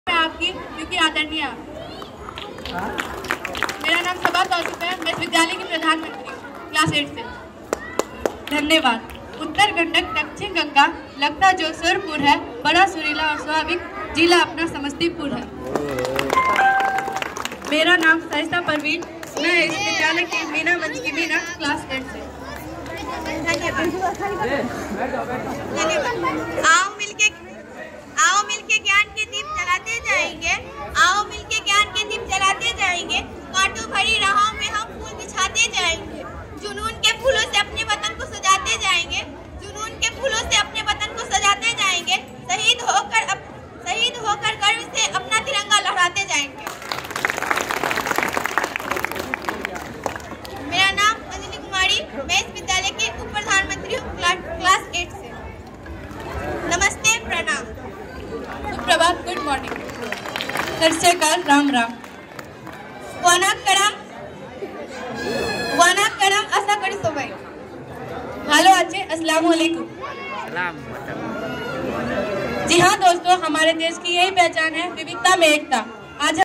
एंकर की भूमिका में स्कूली बच्चे
@achoring by the students on the occasion of republic day 2023

@मंच संचालन की भूमिका में छात्राएं

@गणतंत्र दिवस समारोह